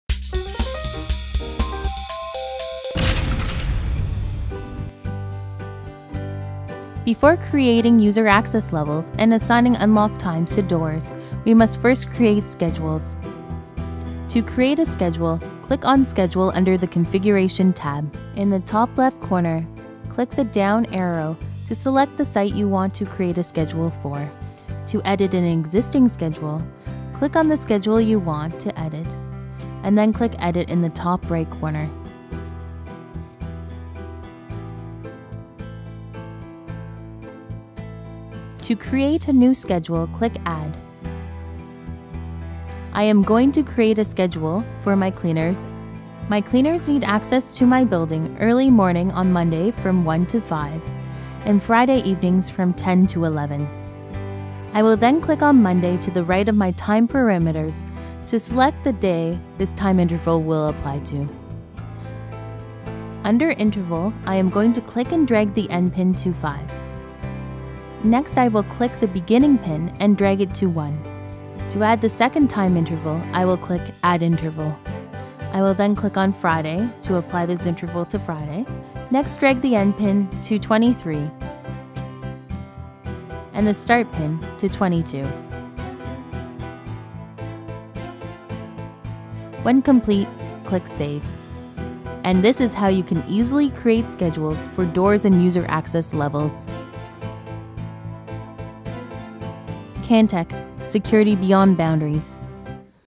EntraPass web Training - Schedules
EntraPass web is a web delivered application that allows administrators to easily perform a host of functions including telephone entry, card management, door control, live video, maps, alarm management and much more. It brings simplicity to security professionals wherever they are, and allows them greater accessibility to handle those important, day-to-day access-related tasks. EntraPass web features a user-centered design that makes it simple to understand and navigate and enhances the effectiveness of the security system, allowing users to better protect those assets that are most important to them. Equally important is the scalability of EntraPass web, which can grow as the systems it supports are upgraded or expanded.